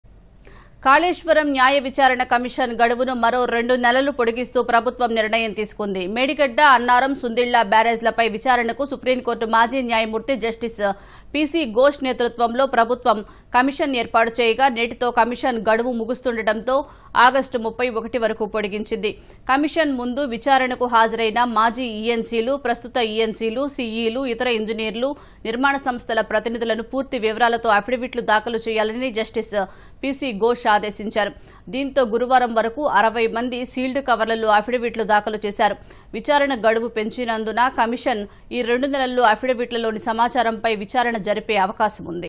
కాళేశ్వరం న్యాయ విచారణ కమిషన్ గడువు మరో రెండు నెలలు పొడిగింపు | Justice PC Ghose commission
కాళేశ్వరం న్యాయ విచారణ కమిషన్ గడువును మరో రెండు నెలలు పొడిగిస్తూ ప్రభుత్వం నిర్ణయం తీసుకుంది. మేడిగడ్డ, అన్నారం, సుందిళ్ల, బ్యారేజీలపై విచారణకు సుప్రీంకోర్టు మాజీ న్యాయమూర్తి జస్టిస్  పీసీ ఘోష్  నేతృత్వంలో ప్రభుత్వం కమిషన్  ఏర్పాటుచేయగా... నేటితో కమిషన్  గడువు ముగుస్తుండటంతో ఆగస్టు 31 వరకు పొడిగించింది.